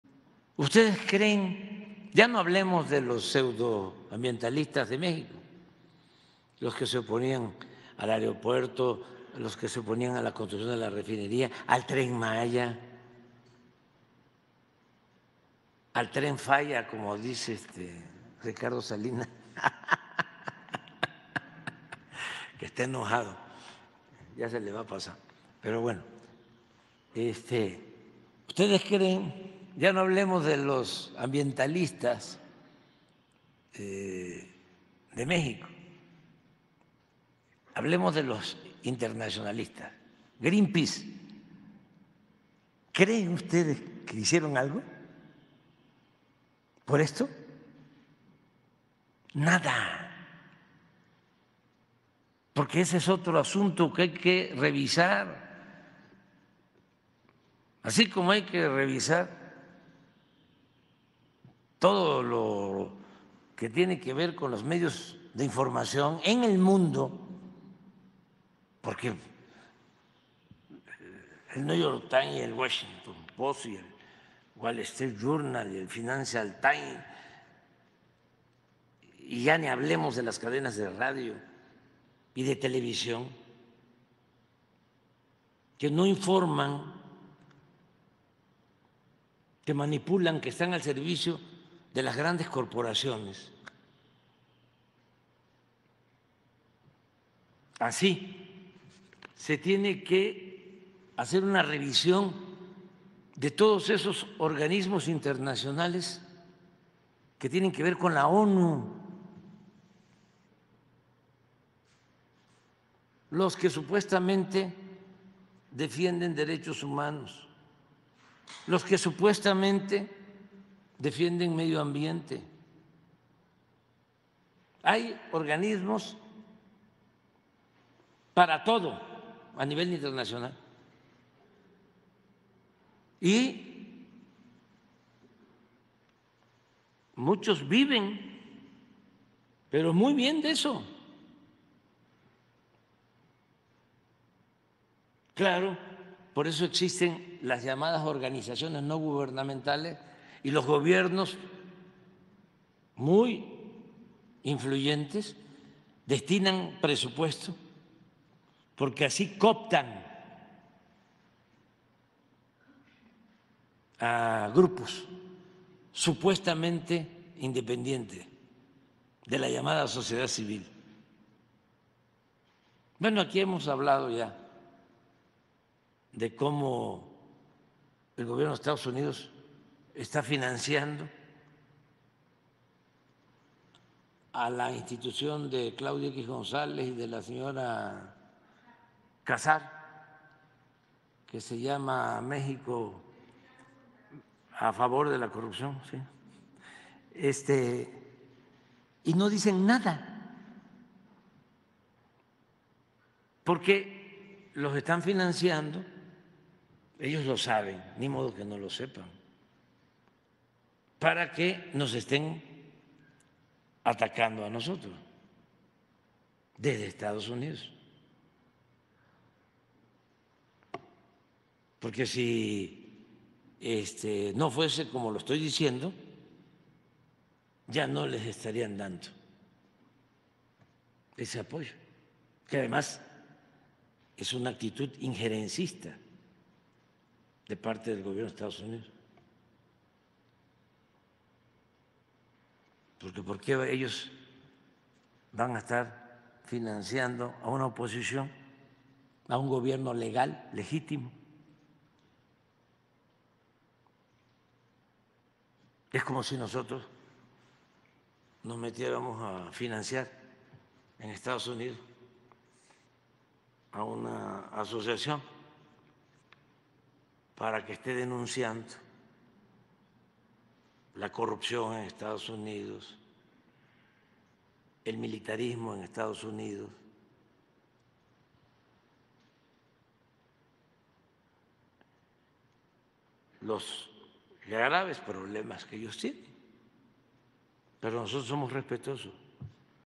AMLO SE BURLA DE SALINAS PLIEGO POR DECIR “TREN FALLA”... DICE QUE YA SE LE VA A PASAR
El presidente López Obrador se carcajeó a mitad de su mañanera al decir que el magnate y barbaján Ricardo Salinas Pliego le llama al Tren Maya el “Tren Falla”... El mandatario terminó de humillar al “dientes de burro”, diciendo que es tan básico que “ya se le va a pasar”.